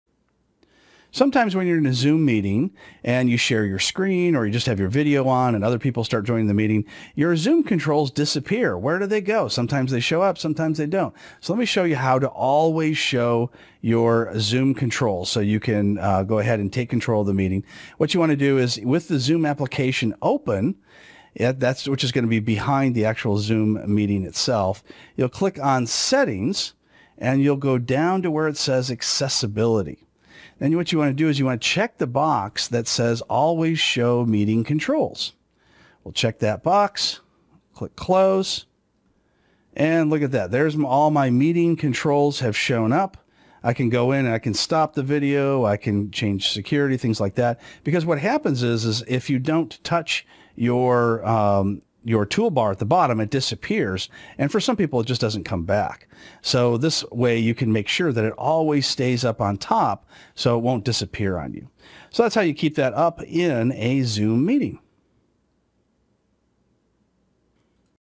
How to keep the Zoom tools from disappearing
Professor Robert McMillen shows you how to keep the Zoom meeting tools always visible. They tend to disappear when you don't click on them and sometimes won't come back.